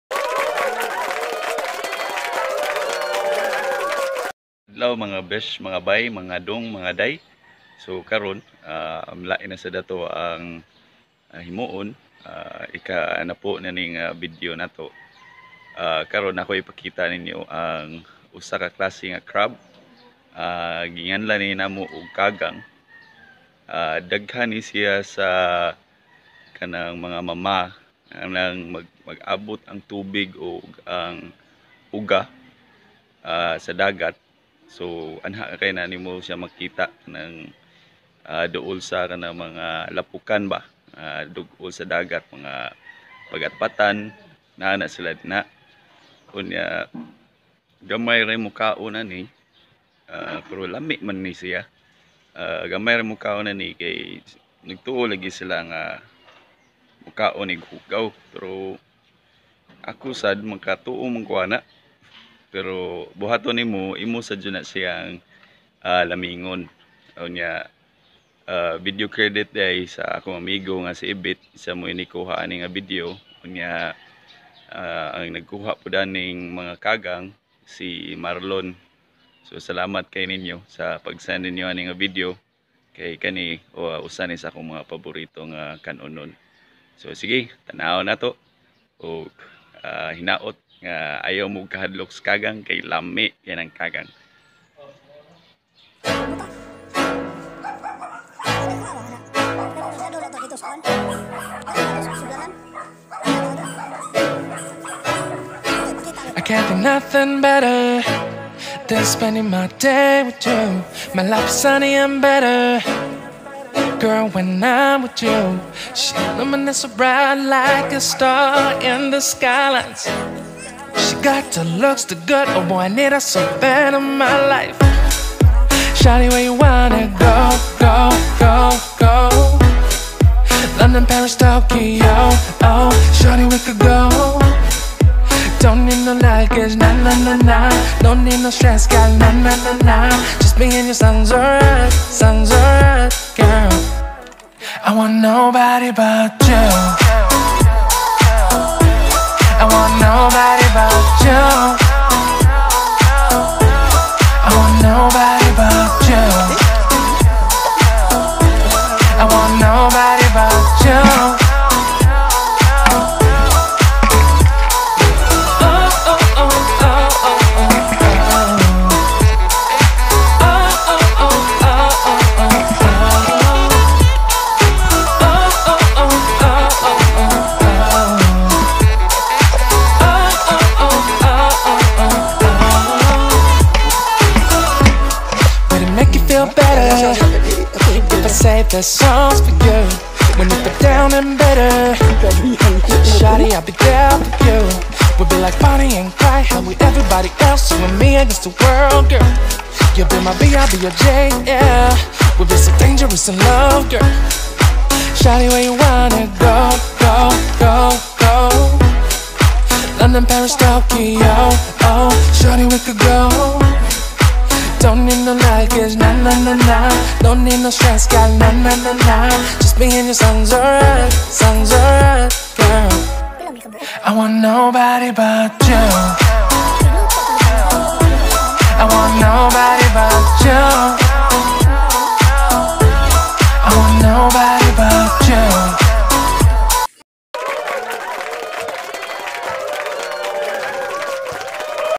How to catch a Crab | kagang | Basketball Travels23
How to catch a crab | kagang | gikuha gamit ang pas-ong. Catch a crab with a bamboo trap.